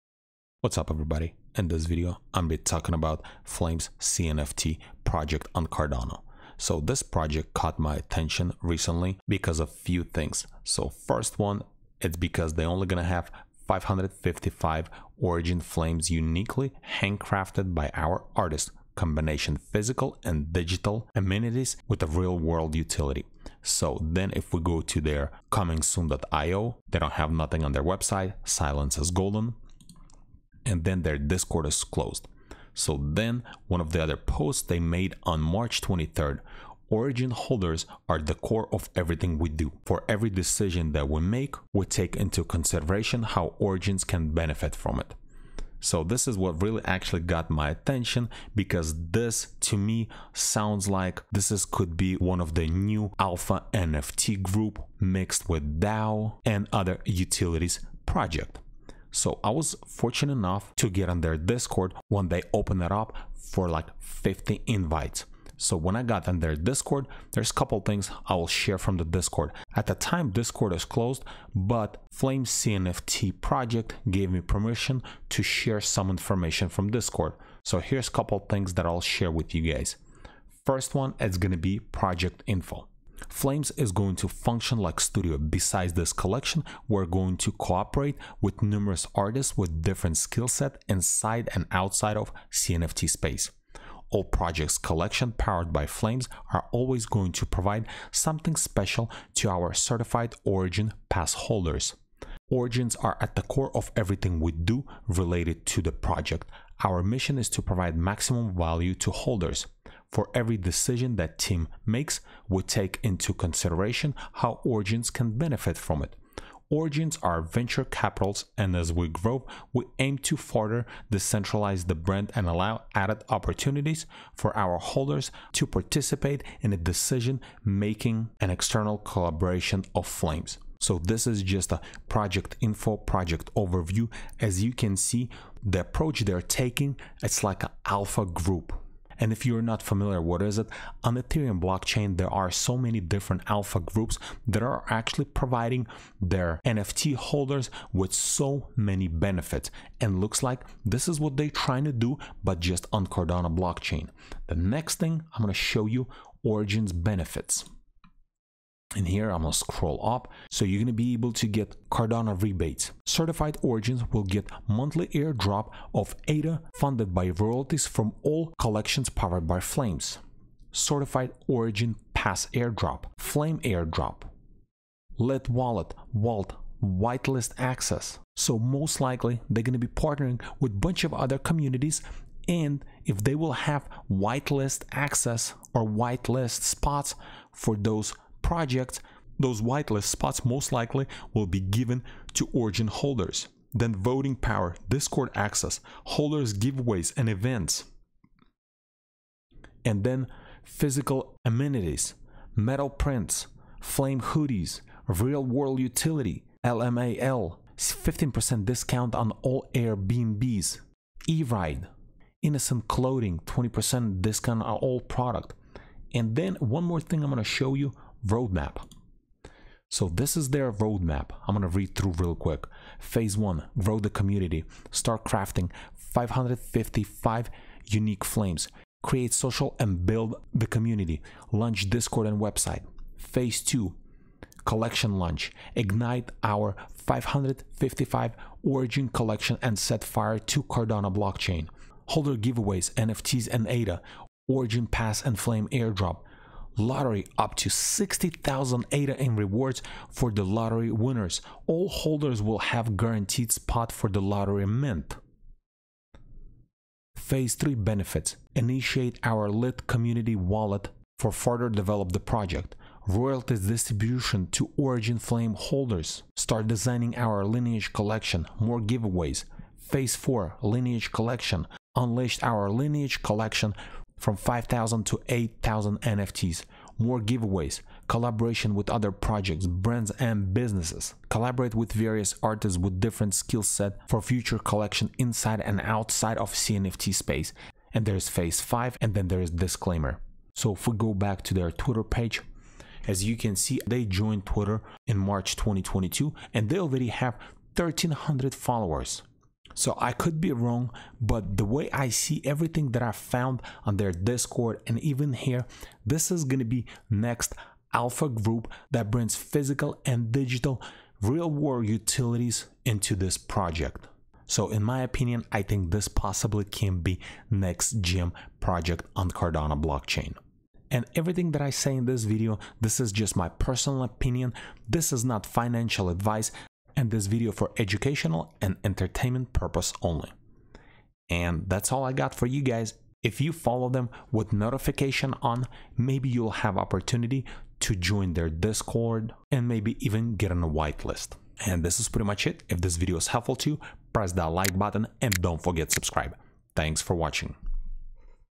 FlamesCNFT - Alpha Group on Cardano
New Alpha Group on Cardano BlockChain with real word utilities and a lot of pass holder benefits.

PERSONAL GEAR
4k Asus Monitor
Video Camera Tripod